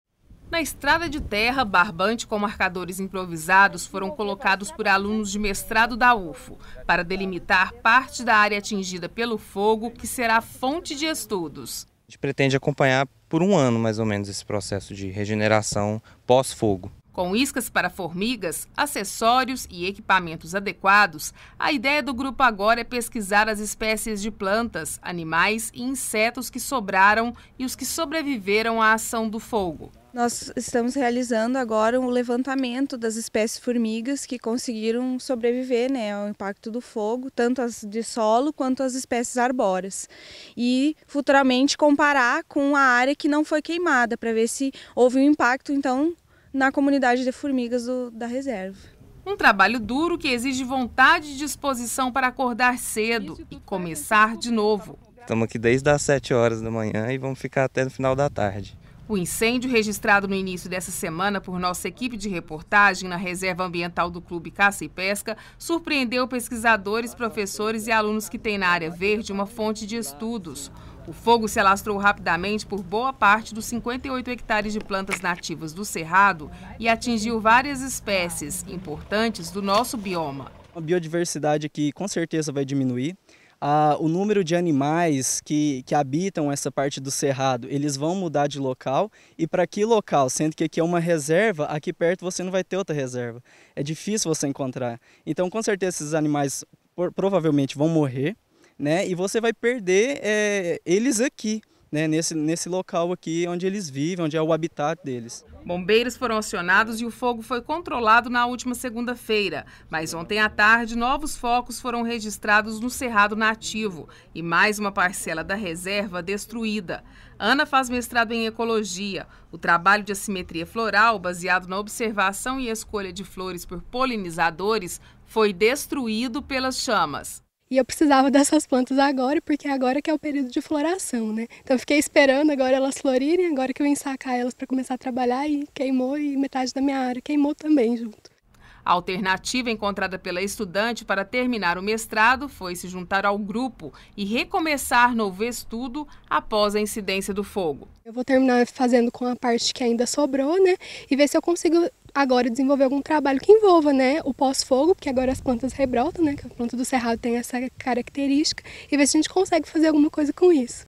RESCALDO INCÊNDIO matéria exibida em 08/10/2014
Depois do incêndio que atingiu parcialmente a reserva ambiental do clube Caça e Pesca, universitários de cursos de graduação, pós graduação e mestrado da UFU, fazem o levantamento da área atingida e recomeçam novas pesquisas sobre as espécies que sobreviveram ao fogo.